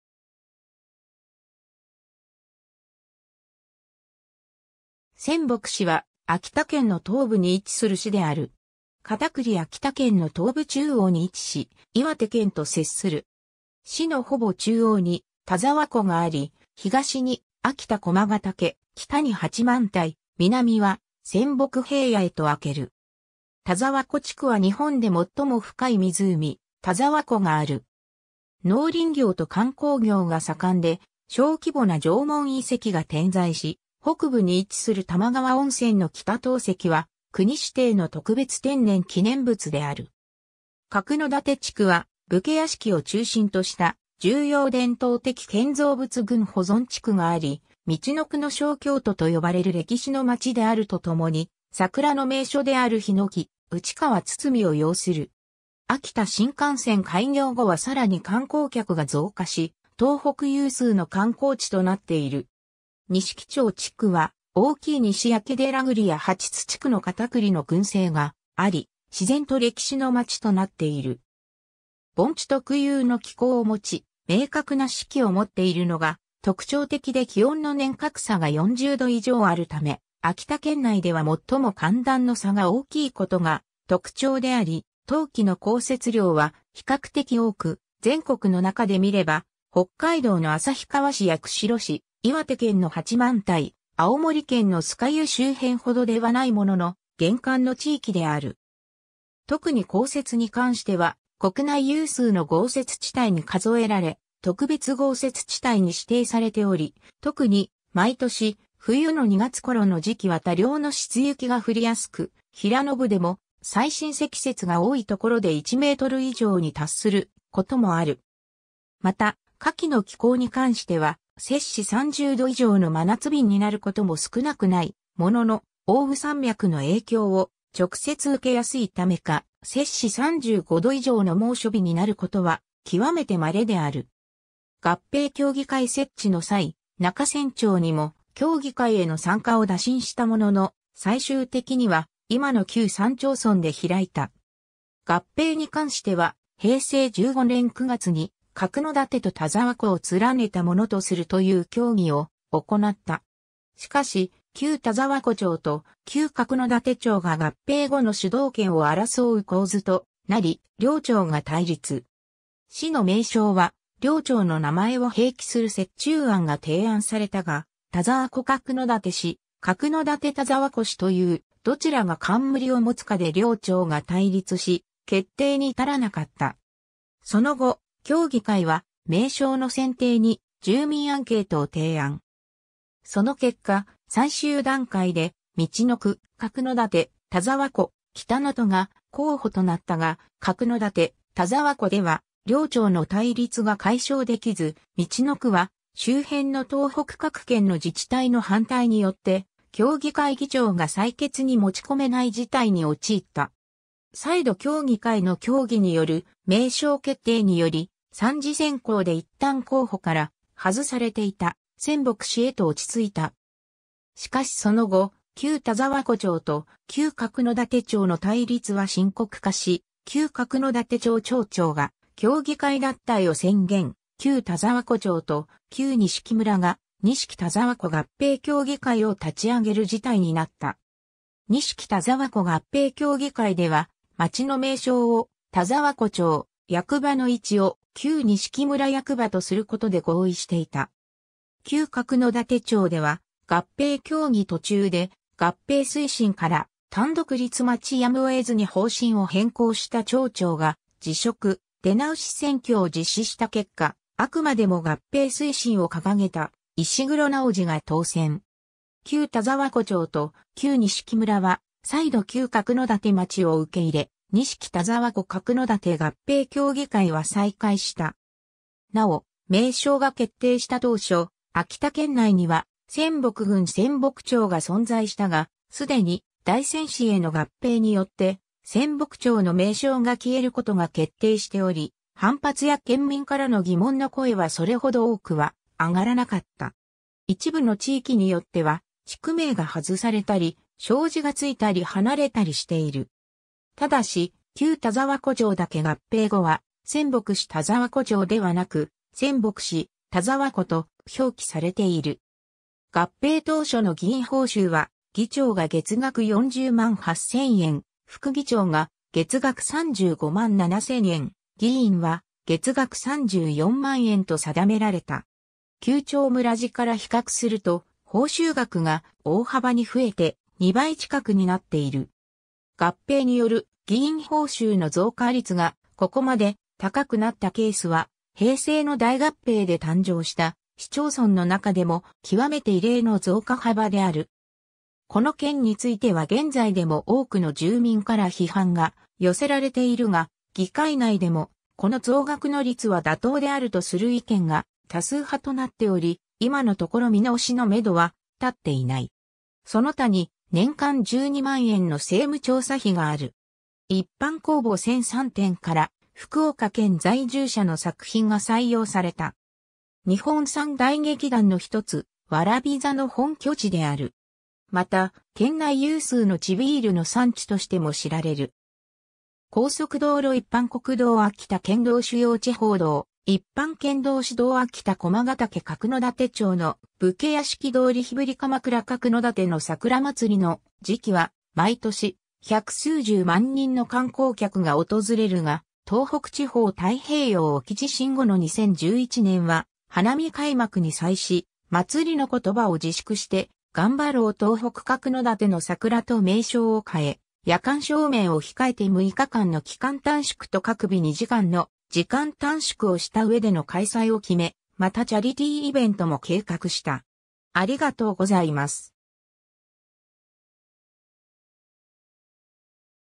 仙北市
仙北市（せんぼくし）は、秋田県の東部に位置する市である。
 カタクリ（仙北市で撮影） 秋田県の東部中央に位置し、岩手県と接する。
市のほぼ中央に田沢湖があり、東に秋田駒ヶ岳、北に八幡平、南は仙北平野へと開ける。
 田沢湖地区は日本で最も深い湖・田沢湖がある。
農林業と観光業が盛んで、小規模な縄文遺跡が点在し、北部に位置する玉川温泉の北投石は国指定の特別天然記念物である。
角館地区は武家屋敷を中心とした重要伝統的建造物群保存地区があり、「みちのくの小京都」と呼ばれる歴史の町であるとともに、桜の名所である桧木内川堤を擁する。
秋田新幹線開業後は更に観光客が増加し、東北有数の観光地となっている。
 西木町地区は、大きい西明寺栗や八津地区のカタクリの群生があり、自然と歴史の町となっている。
 盆地特有の気候を持ち、明確な四季を持っているのが特徴的で気温の年較差が40度以上あるため秋田県内では最も寒暖の差が大きいことが特徴であり、冬季の降雪量は比較的多く、全国の中で見れば北海道の旭川市や釧路市、岩手県の八幡平、青森県の酸ヶ湯周辺程ではないものの、厳寒の地域である。
特に降雪に関しては国内有数の豪雪地帯に数えられ、特別豪雪地帯に指定されており、特に毎年、冬の2月頃の時期は多量の湿雪が降りやすく、平野部でも最深積雪が多い所で1m以上に達することもある。
また、夏季の気候に関しては30℃以上の真夏日になることも少なくないものの、奥羽山脈の影響を直接受けやすいためか35℃以上の猛暑日になることは極めて稀である。
 合併協議会設置の際、中仙町（現：大仙市中仙地区）にも協議会への参加を打診したものの、最終的には今の旧3町村で開いた。
 合併に関しては平成15年9月に「角館と田沢湖を連ねたものとする」という協議を行った。
しかし、 旧田沢湖町と旧角館町が合併後の主導権を争う構図となり両町が対立。
市の名称は両町の名前を併記する折衷案が提案されたが、「田沢湖角館市」「角館田沢湖市」という、どちらが冠を持つかで両町が対立し決定に至らなかった。
 その後、協議会は名称の選定に住民アンケートを提案。
その結果、最終段階で「みちのく」・「角館」・「田沢湖」・「北の都」が候補となったが、「角館」「田沢湖」では両町の対立が解消できず、「みちのく」は周辺の東北各県（特に太平洋側）の自治体の反対（「みちのく」は陸奥国のことであり、旧出羽国にあたるこの地区は入っていない）によって、協議会議長が採決に持ち込めない事態に陥った。
再度協議会の協議による名称決定により、3次選考でいったん候補から外されていた「仙北市」へと落ち着いた。
 しかしその後、旧田沢湖町と旧角館町の対立は深刻化し、旧角館町町長が協議会脱退を宣言、旧田沢湖町と旧西木村が「西木田沢湖合併協議会」を立ち上げる事態になった。
「西木田沢湖合併協議会」では町の名称を「田沢湖町」、役場の位置を旧西木村役場とすることで合意していた。
 旧角館町では、合併協議途中で「合併推進」から「単独立町やむを得ず」に方針を変更した町長が辞職、出直し選挙を実施した結果、「あくまでも合併推進」をかかげた石黒直次（初代市長）が当選。
旧田沢湖町と旧西木村は、再度旧角館町を受け入れ「西木田沢湖角館合併協議会」は再開した（合併までの間「西木田沢湖角館合併協議会」「西木田沢湖合併協」が並存した）。
 なお、名称が決定した当初、秋田県内には仙北郡仙北町が存在したが、すでに大仙市への合併によって、仙北町の名称が消えることが決定しており、反発や県民からの疑問の声はそれほど多くは上がらなかった。
 一部の地域によっては、地区名がはずされたり、小字がついたり離れたりしている。
但し、旧田沢湖町だけ合併後は「仙北市田沢湖町」ではなく「仙北市田沢湖」と表記されている。
 合併当初の議員報酬は議長が月額408,000円、副議長が月額357,000円、議員は月額340,000円と定められた。
旧町村時から比較すると、報酬額が大幅に増えて、2倍近くになっている。
合併による議員報酬の増加率がここまで高くなったケースは平成の大合併で誕生した市町村の中でも極めて異例の増加幅である。
この件については現在でも多くの住民から批判が寄せられているが、議会内でもこの増額の率は妥当であるとする意見が多数派となっており、今のところ見直しの目処は立っていない。
 その他に年間12万円の政務調査費がある。
 一般公募1003点から福岡県在住（当時）者の作品が採用...